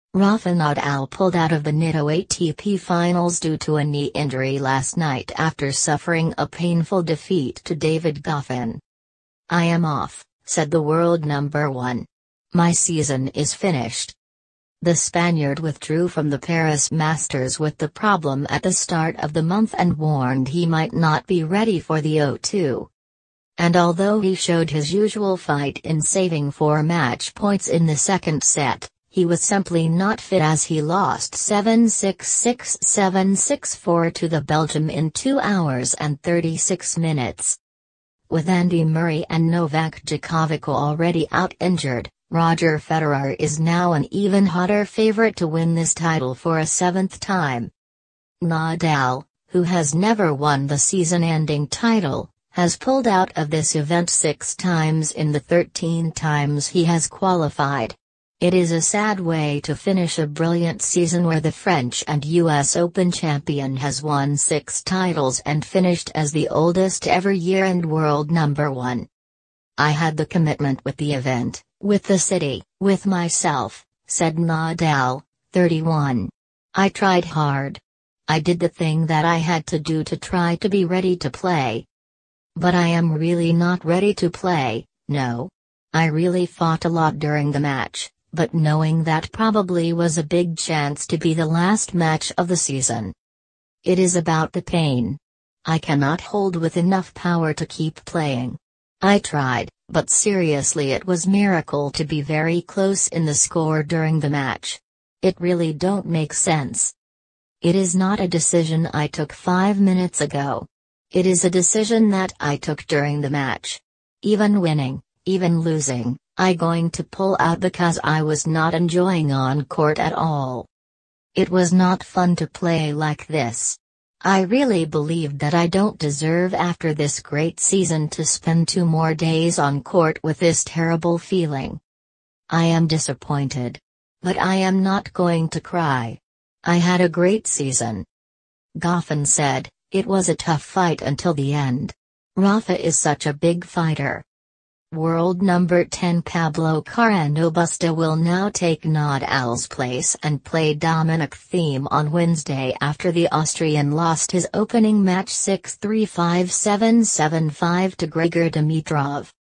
Rafa Nadal withdraws from ATP World Tour finals as world No.1 succumbs to knee injury in defeat by D
Rafa Nadal pulled out of the Nitto ATP Finals due to a knee injury last night after suffering a painful defeat to David Goffin.
“I am off,” said the world No.1. “My season is finished.”
The Spaniard withdrew from the Paris Masters with the problem at the start of the month and warned he might not be ready for The O2.